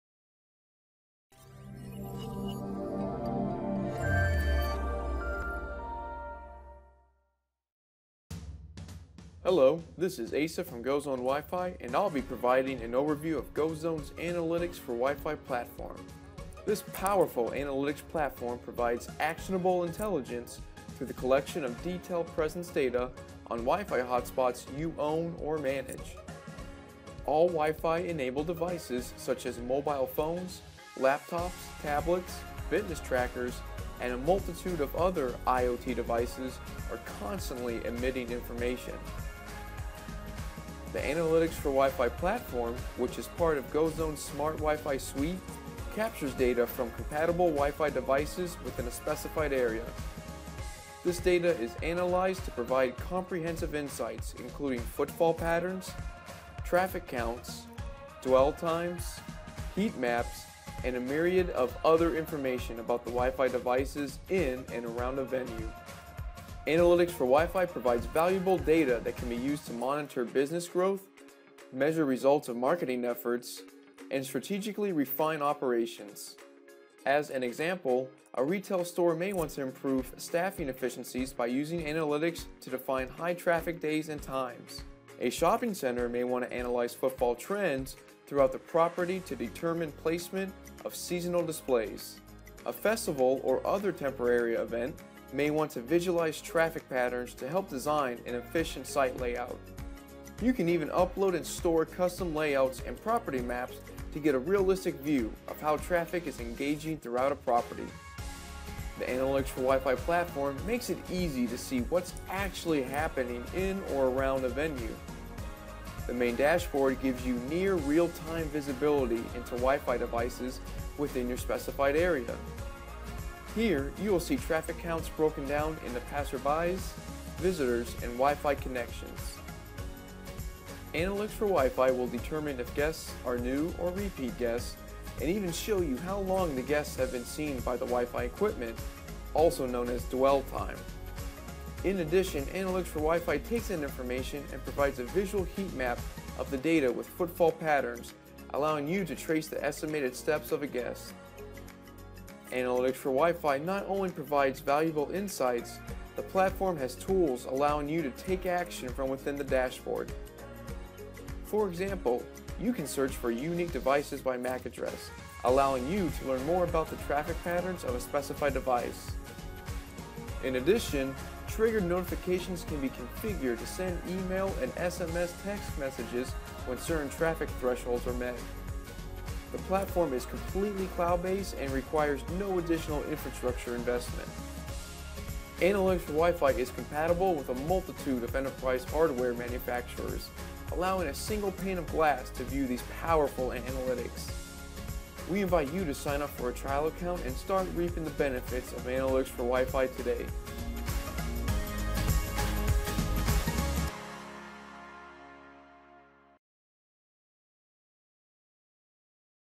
Analytics4WiFi Overview
Powerful location analytics powered by your guest WiFi. Analytics4WiFi captures and analyzes where your customers are, and how they move through and interact with your space - all powered by guest WiFi.